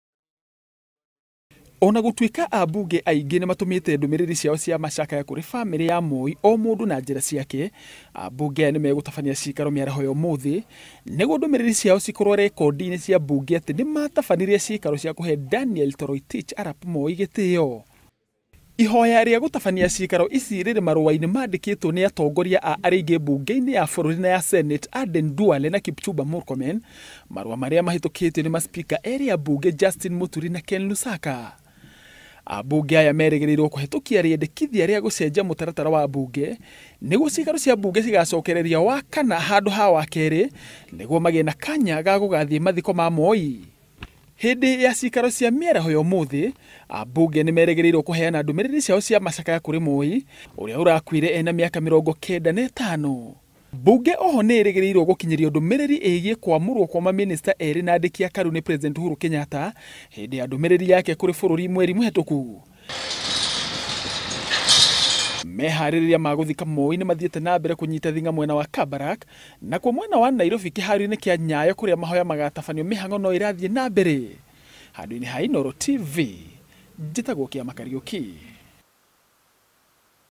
Mbunge ya Senate na ya bururi gucokereria cikaro gucakaya
Mbunge ya bururi na iria ya senate nicigucokereria cikaro miaraho ya umuthi, niguo ambunge magii na kanya ga gucakaya uria waari presidentr wa keeri Daniel Arap Moi...Cikaro ici ciititio ni spika wa mbunge Justin Muturi